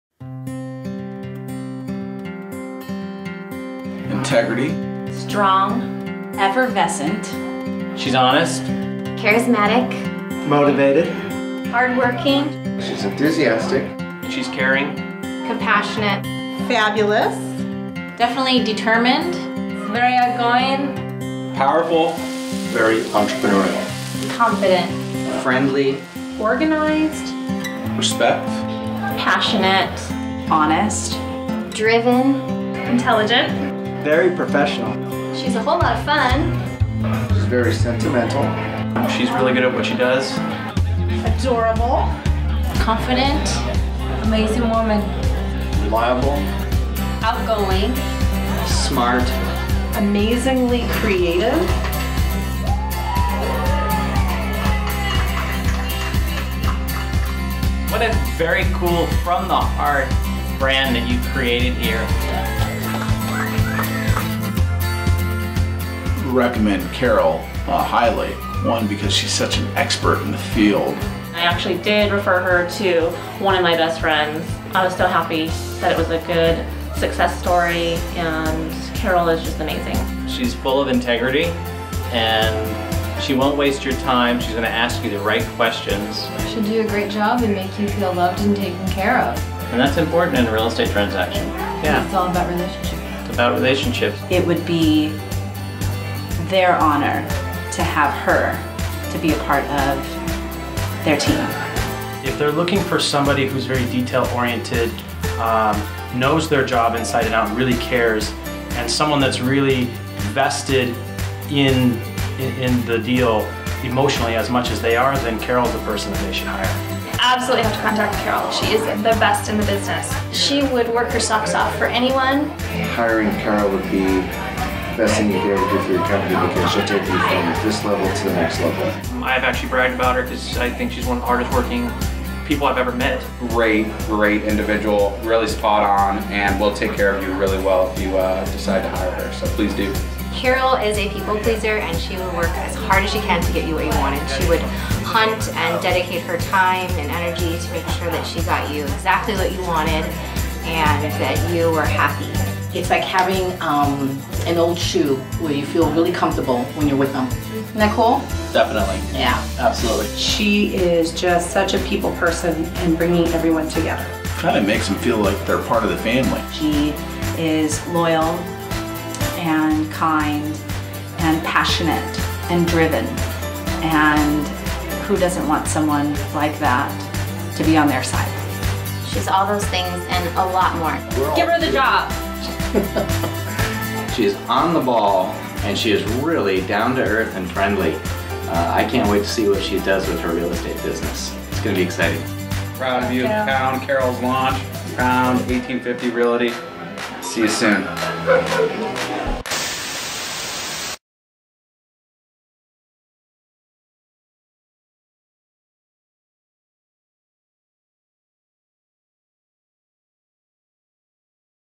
1850 Realty Launches in San Diego
1850 Realty is a homegrown Real Estate Brokerage that celebrates everything that makes San Diego unique. The official launch party was held on 8/8/13 where clients, friends & colleagues came to show their support for this new venture.